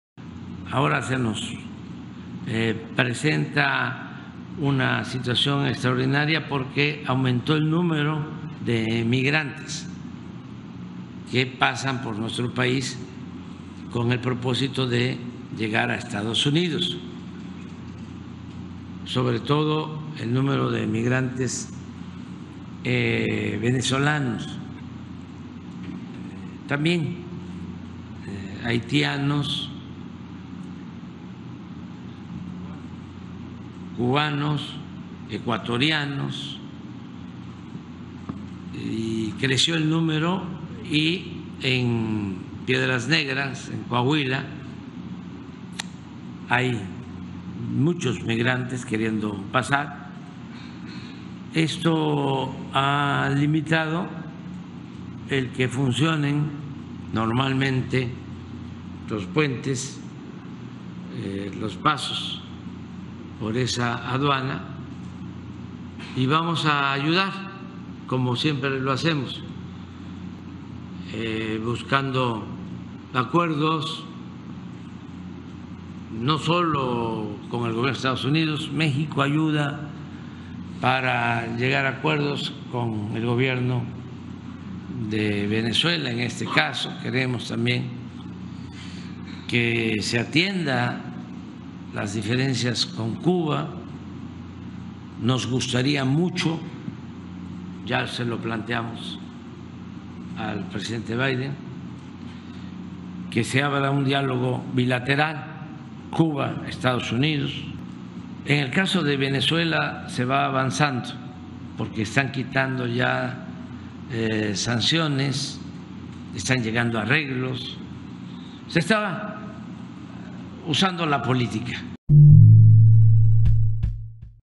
AMLO explica cómo ordenar el flujo migratorio
López Obrador abordó el tema del flujo migratorio y anunció la visita del secretario de Estado de Estados Unidos, Antony Blinken, quien encabezará una delegación de alto nivel para abordar la migración irregular en el continente. El objetivo es reforzar las medidas de contención y ordenar el flujo migratorio de personas que buscan llegar a Estados Unidos. El presidente resaltó la importancia de mantener una estrecha colaboración con el gobierno estadounidense para hacer frente a esta situación extraordinaria.